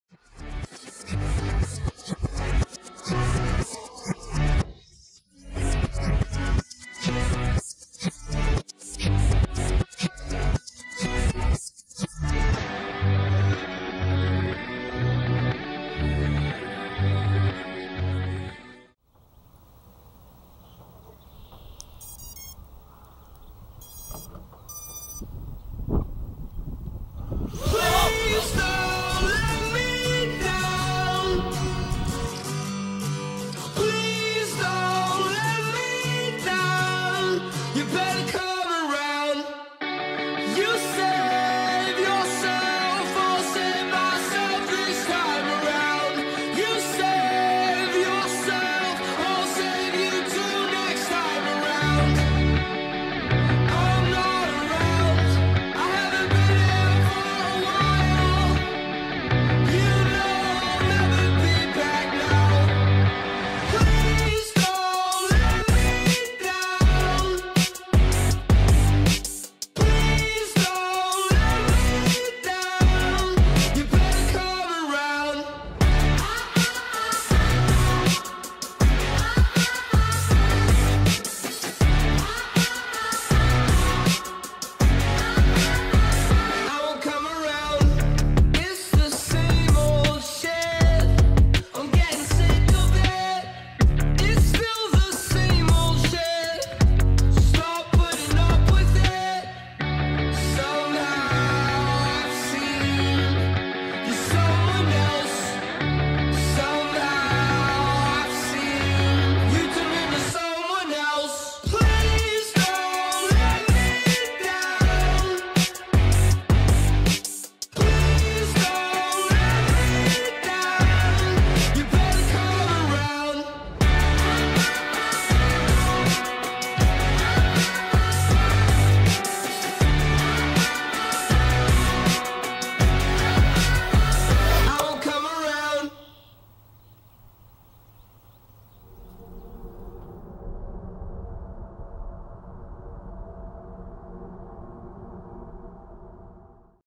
1 Pack in A Windy Park
Wind got up to 18mph gusts today. Heads tested the windy airs with 1 pack at the playground. Little too windy to have any fun. Slow and cautious in this one.

Pilots:

Colin "Heads" (AvantQuads Kira 2.5" Toothpick)